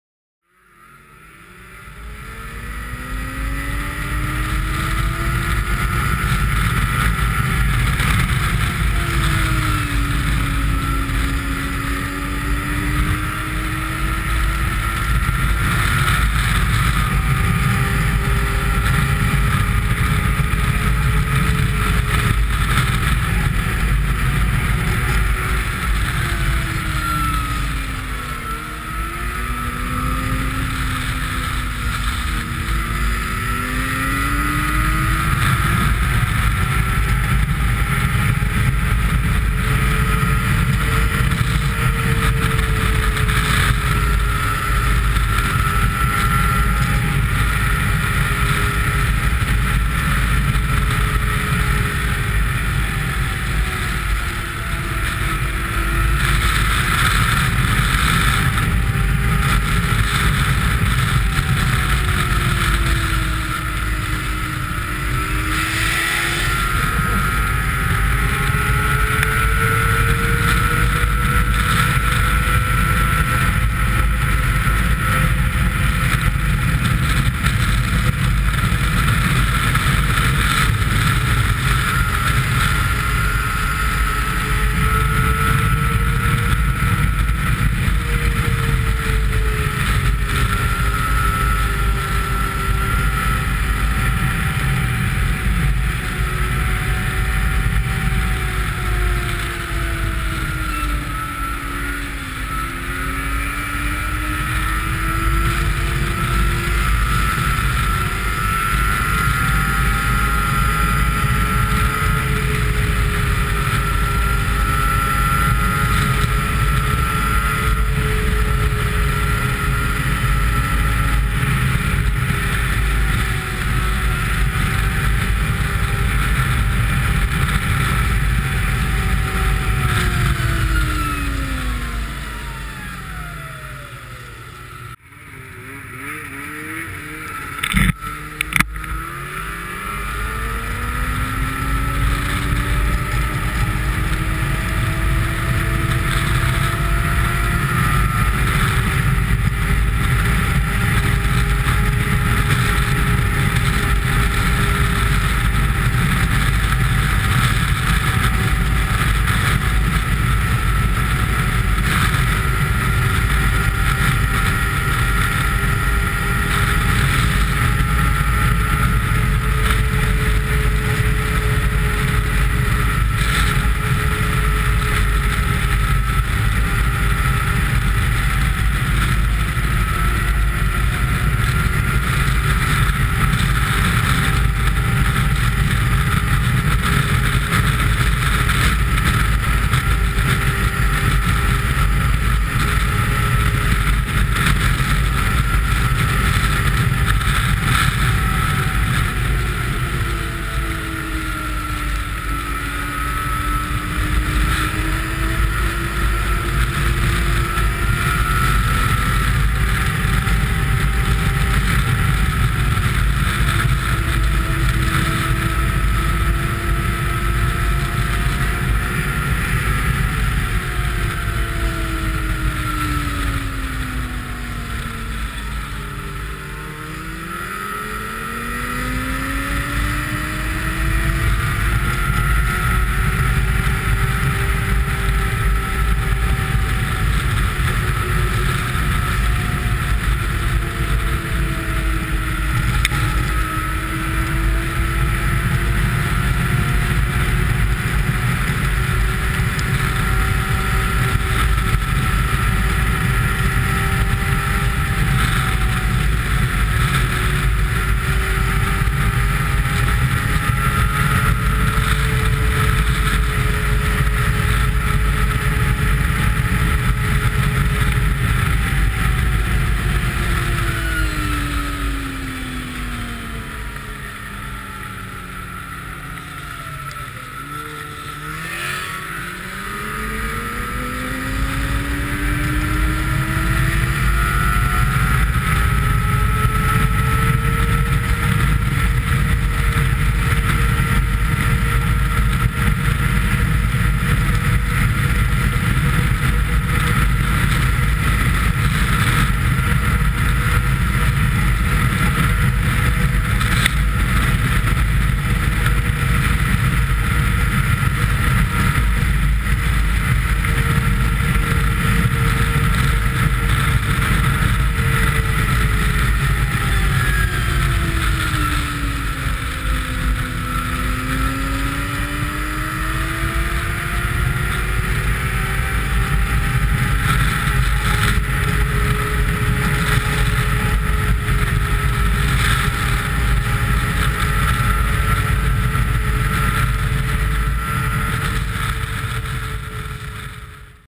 skidoo on my ass
some skidoo on my ass,oh wait its just mike. riding the trails in manitoba.